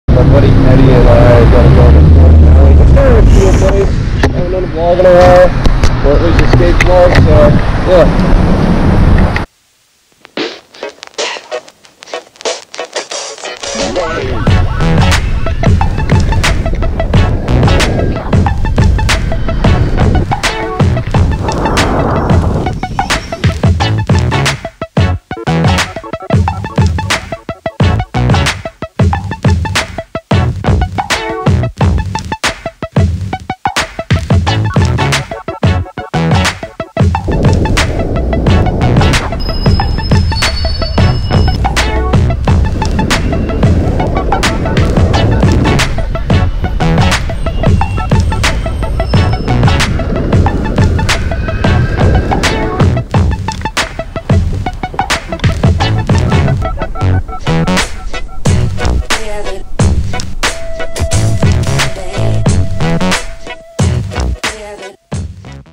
FERN ALLEY| EPISODE#86
A buddy and I had some free time to hang out before he moves across the country. Went to ride down a local street here in the city.